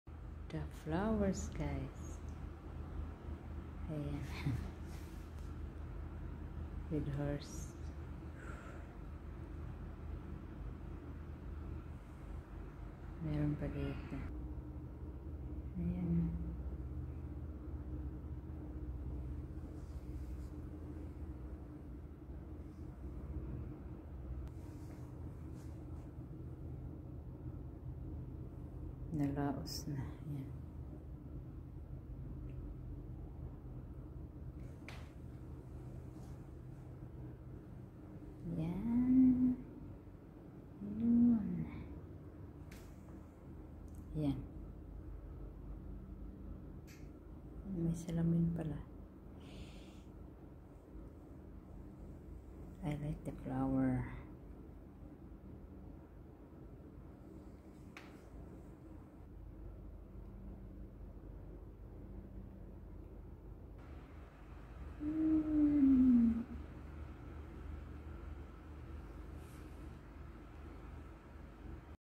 Ang magagandang bulaklak na regalo
Ang mga bulaklak na regalo sa alaga kong newborn baby boy...